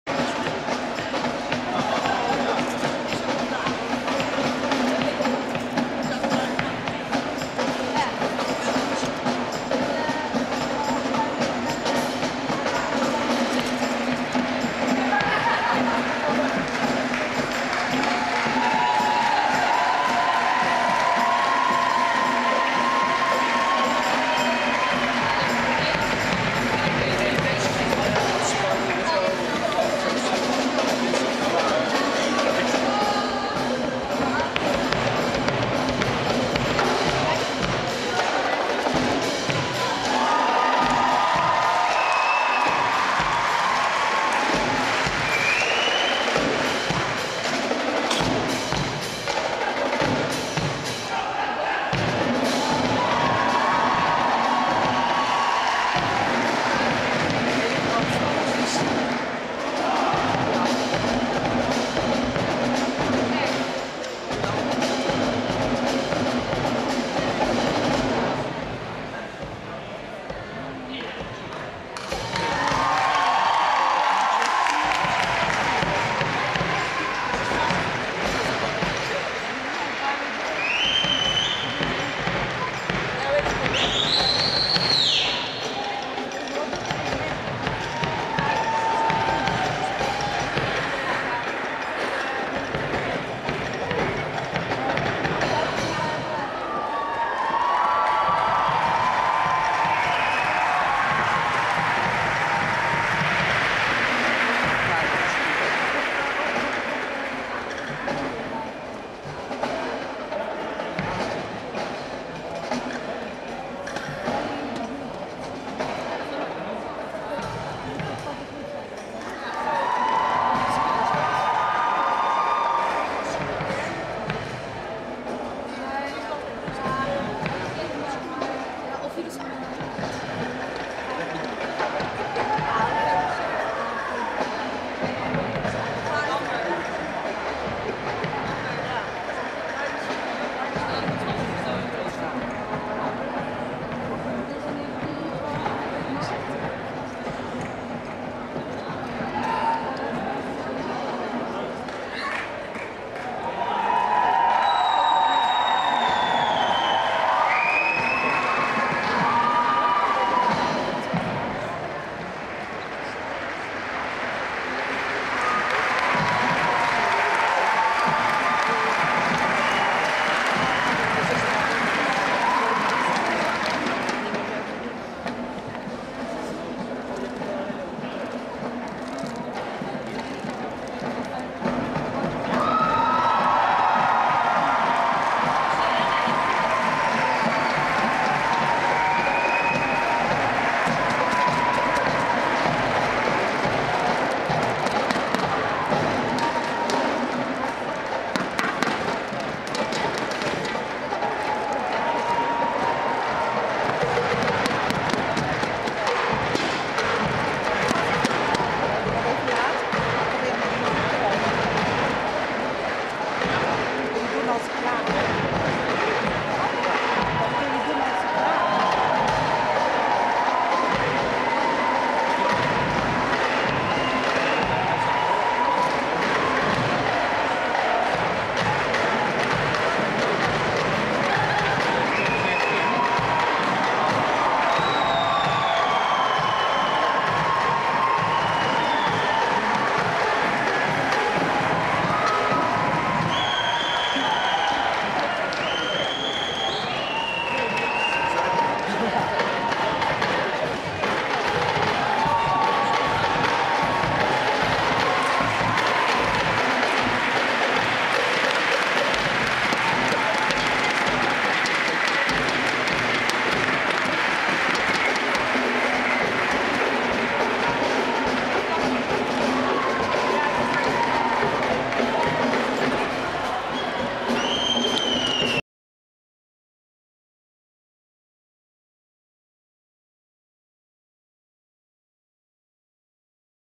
Drumband Concordia Obbicht WMC 2013
Super optreden op het WMC 2013 in Kerkrade.
Sectie Mars First Division.